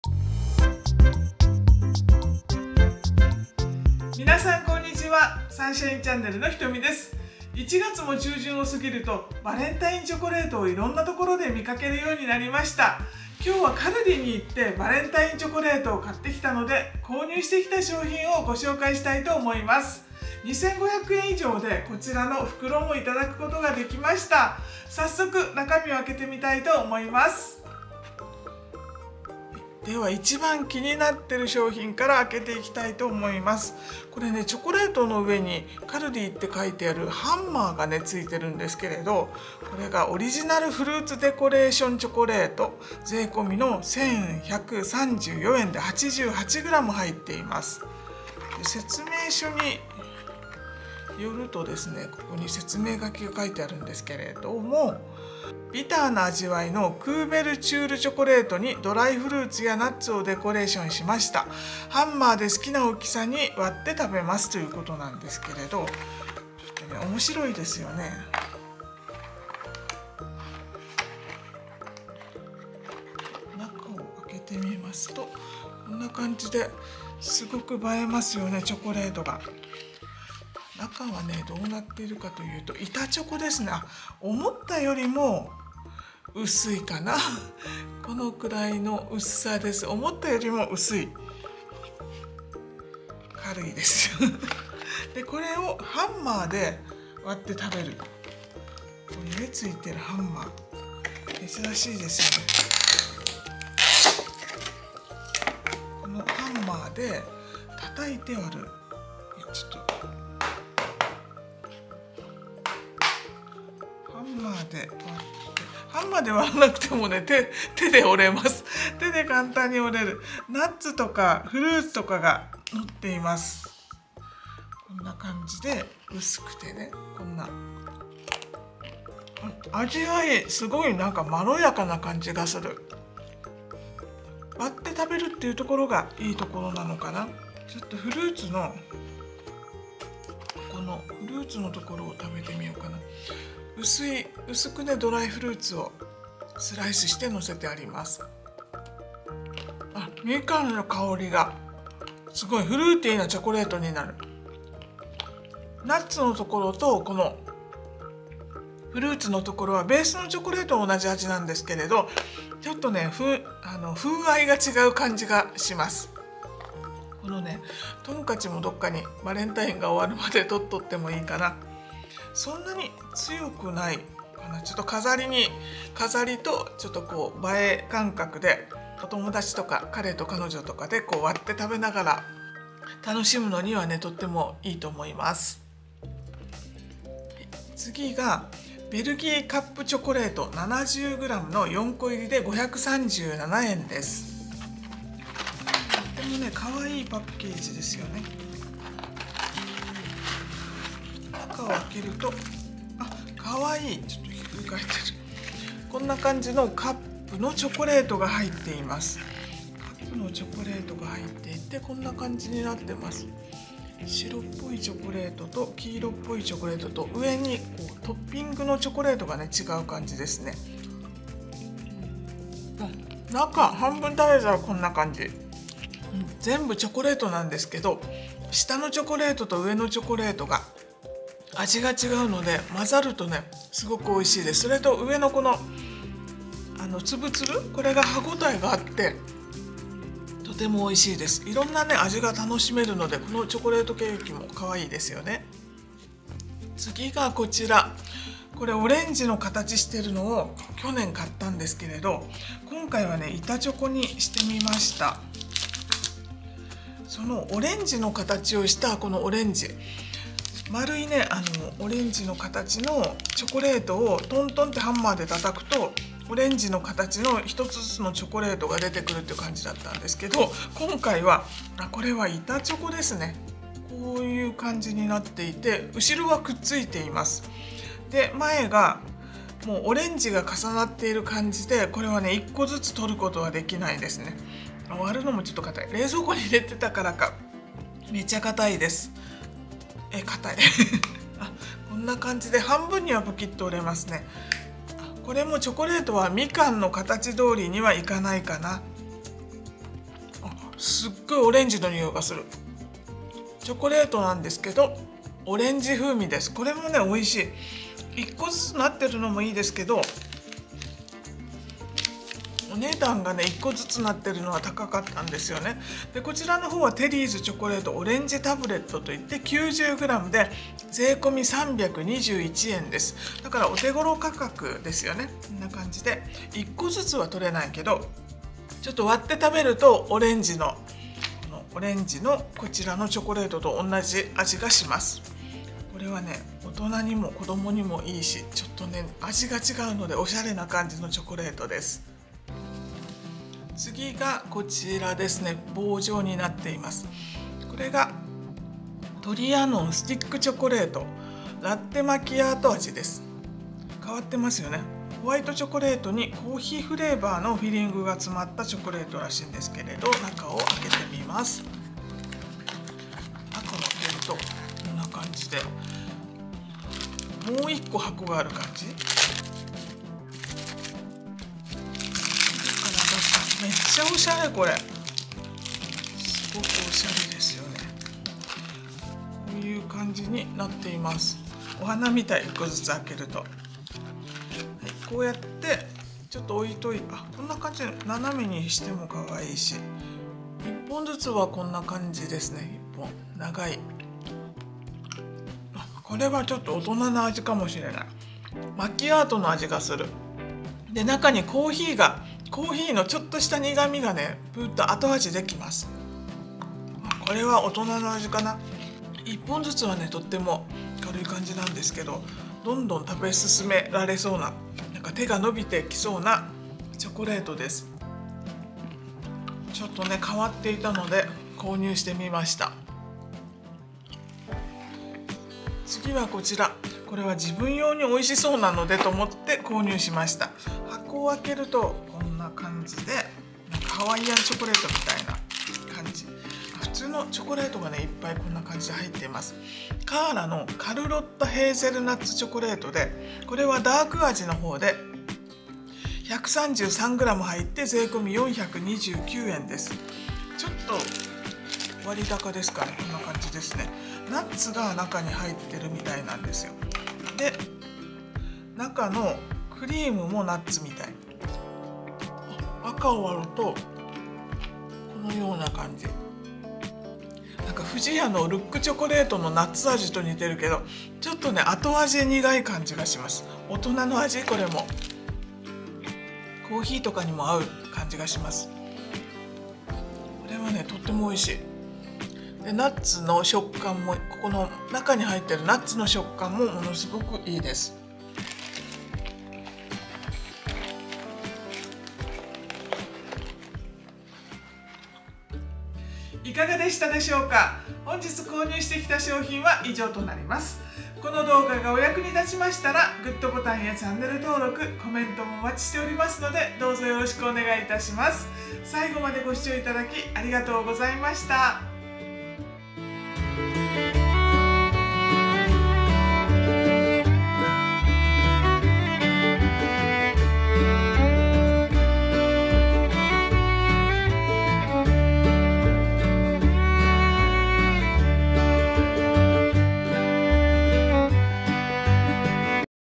【カルディ新商品】バレンタインチョコレート
【カルディ新商品】バレンタインチョコレート2023年1月10日　カルディ購入品

オリジナル フルーツデコレーションチョコレート　88g 税込¥1,134
「ビターな味わいのクーベルチュールチョコレートに、ドライフルーツやナッツなどをデコレーション！
ハンマーでお好みの大きさに割って召し上がれ♪」

テリーズ　チョコレートオレンジ　タブレットミルク　90g　税込321円
「オレンジのフレーバーが人気のテリーズのタブレットタイプです。
高品質のミルクチョコレートとオレンジオイルが配合された爽やかな香りのコンビネーションをお手軽にお楽しみください。」

カーラ　カルロッタ　ヘーゼルナッツチョコレート（ダーク）　133g　税込429円
「濃厚なヘーゼルナッツクリームにヘーゼルナッツをホールのまま入れ、全体をダークチョコで覆いました。」

トリアノン　スティックチョコレート　ラッテマキアート　125g　税込365円
「ホワイトチョコレートにコーヒーフレーバーのフィリングが詰まったスティックタイプのチョコレートです。」

Belgian カップケーキチョコレート 70g 税込537円
「カップケーキの形をしたチョコレートです。
クリームも飾りの部分も全てチョコレートでできています」